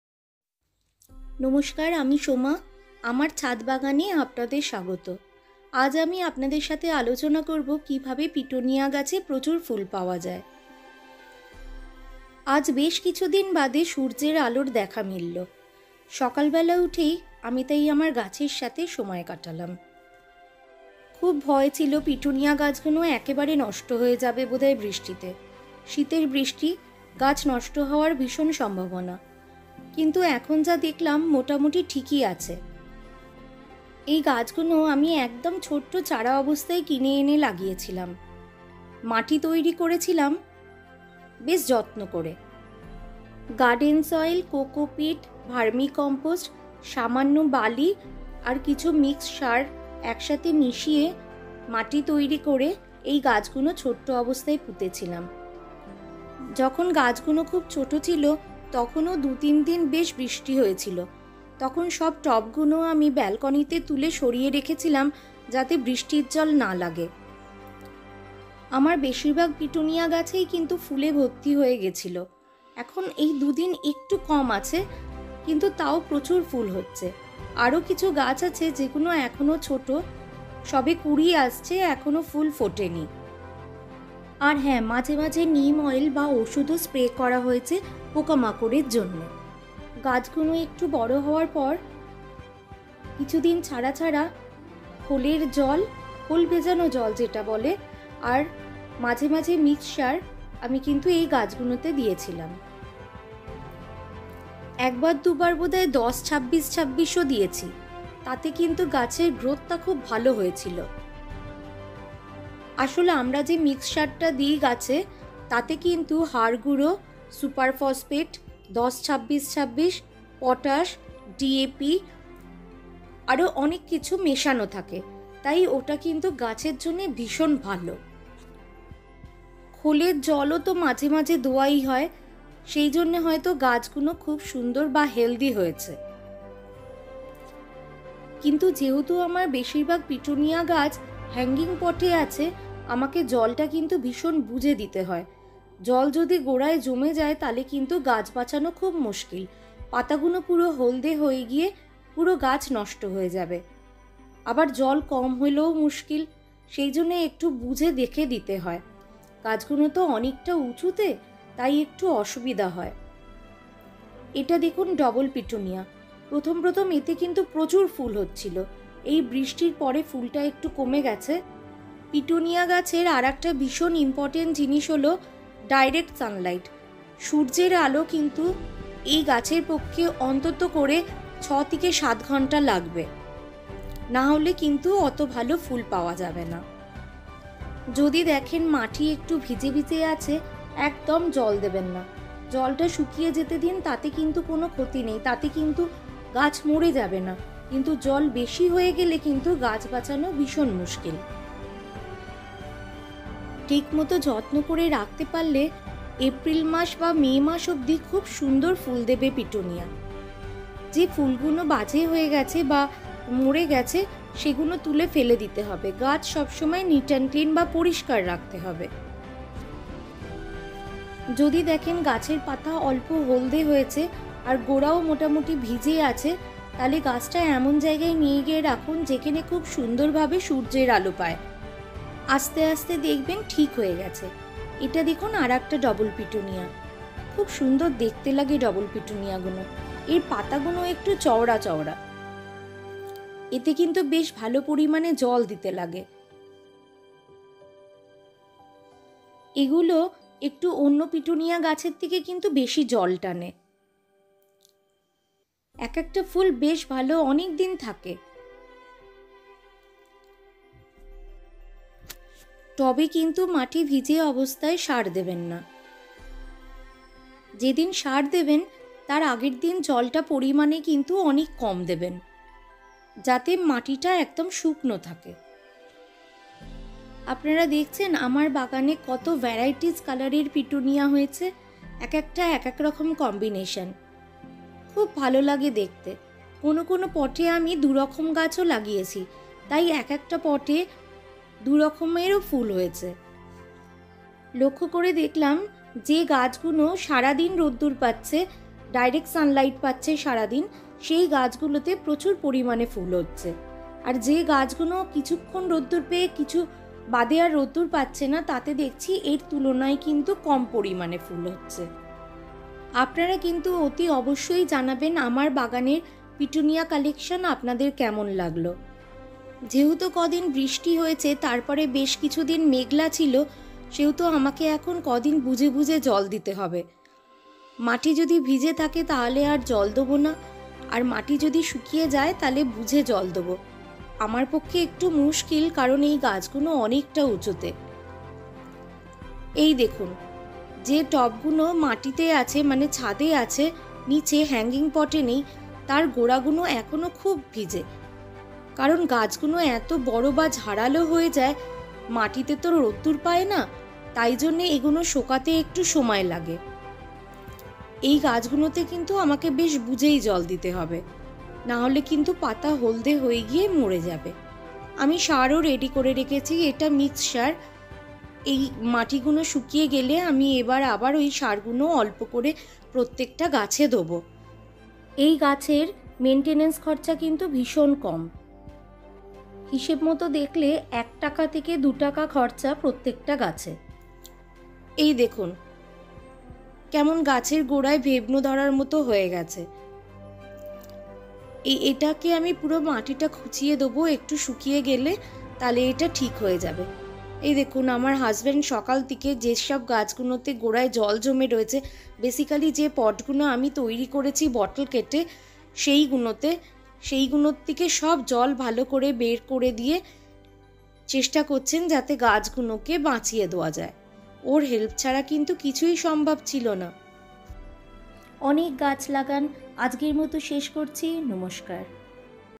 ছাদ বাগান এ পান প্রচুর পিটুনিয়া সামান্য যত্ন করে || মাত্র ১ টাকা খরচ করে
Hello Friends... Today I will share with you how to take care of Petunia plants.